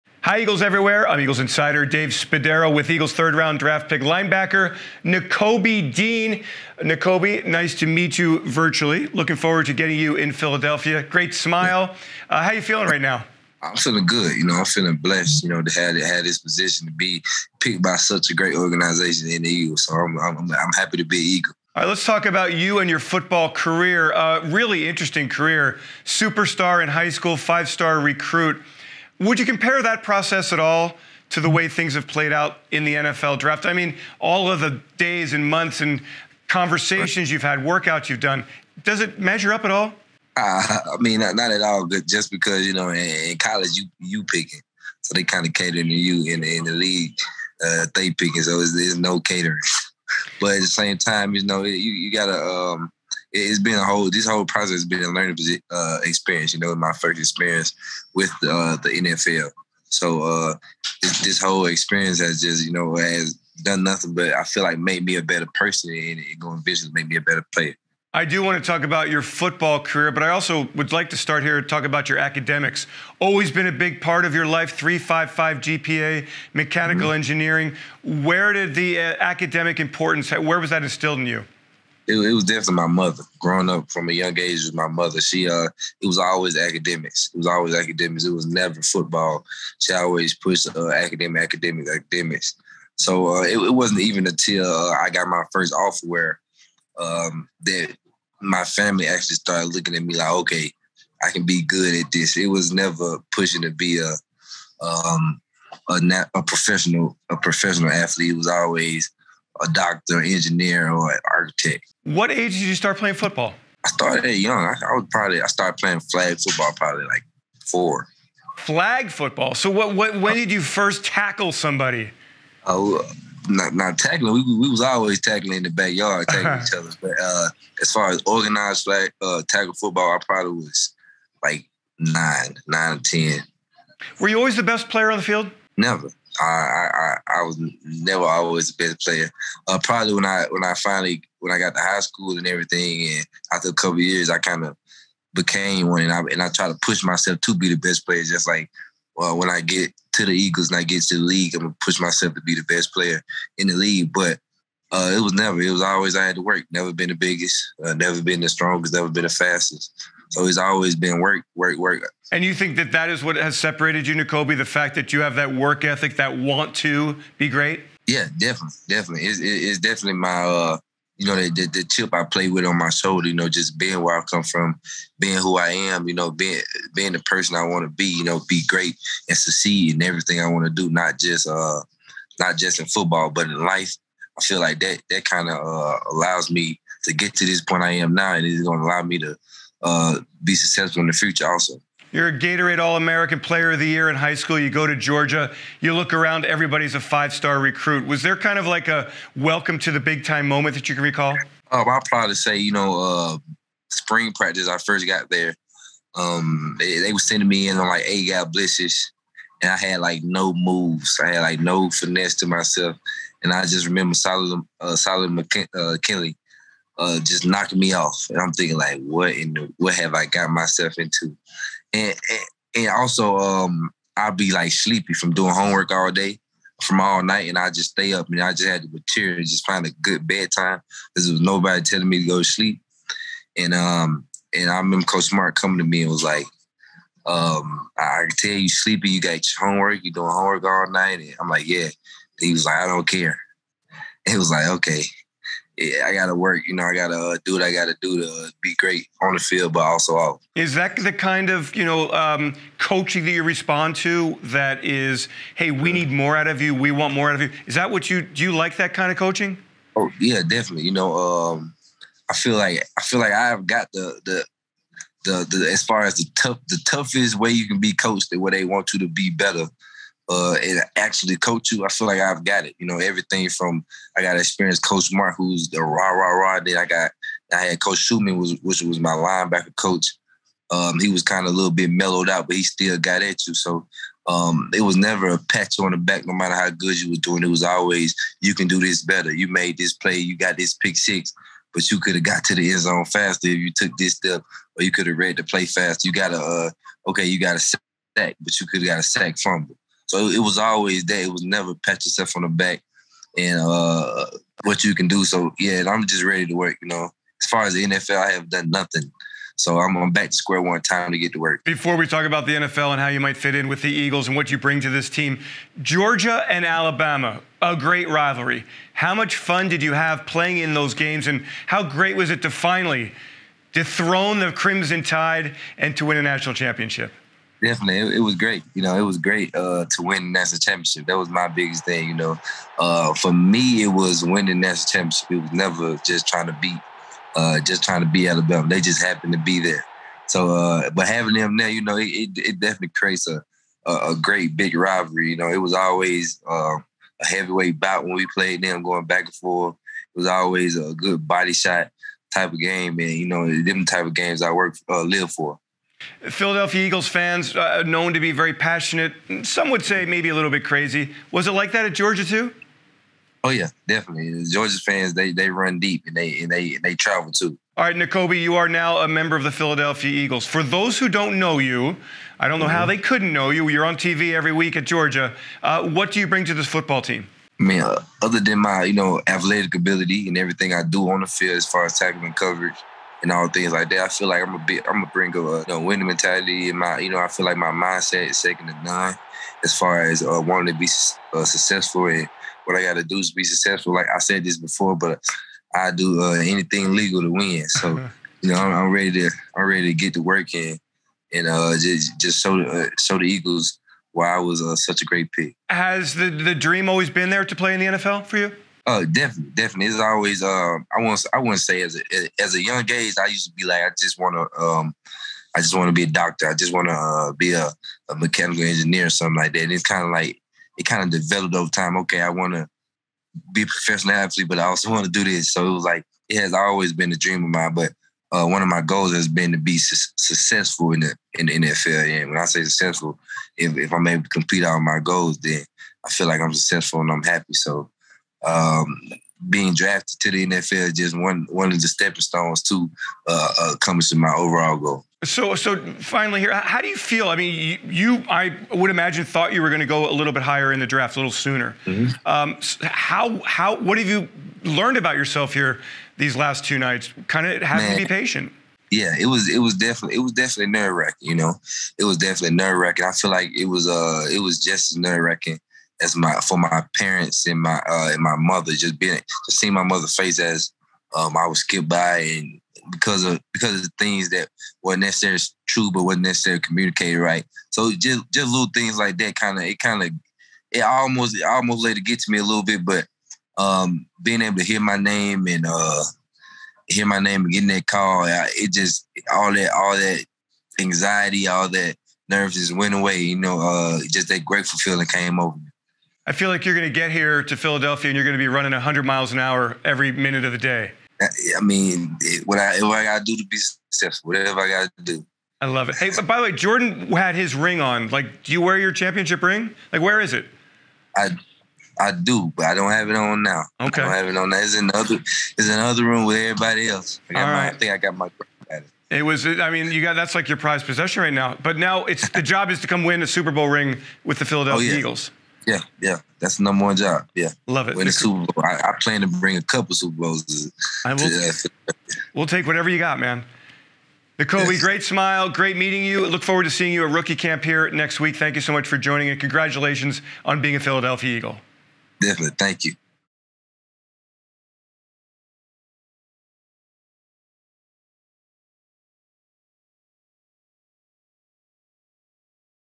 Get to Know Philadelphia Eagles Linebacker Nakobe Dean | Eagles One-on-One
Shortly after being selected by the Philadelphia Eagles in Round 3 of the 2022 NFL Draft, linebacker Nakobe Dean joined Eagles Insider Dave Spadaro to share his excitement about coming to Philadelphia and reuniting with defensive tackle Jordan Davis.